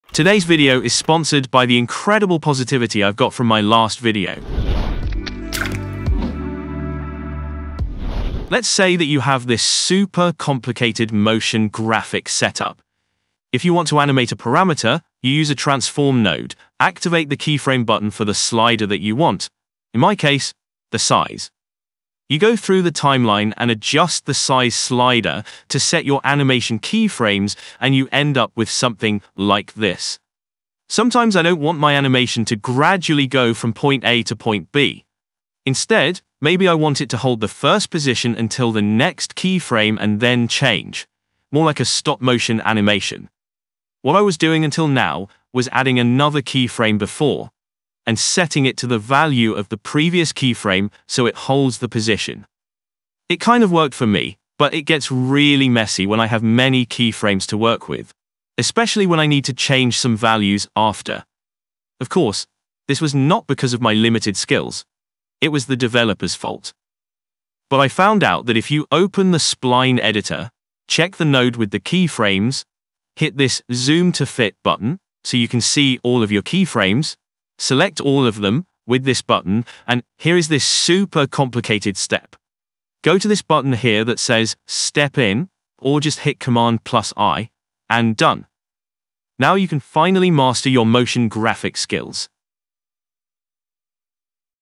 Motion Graphic Tricks #6 | Keyframe Hold | DaVinci Resolve
How to hold the keyframe value in Davinci Resolve Fusion page.

You can send them here :

BTC : 1Ecg6F8UnFHnVxsKhbkecqtge11vBhqS5
ETH: 0xB643C1C8B6C75bcB3426F40f8514CB89Fc294abC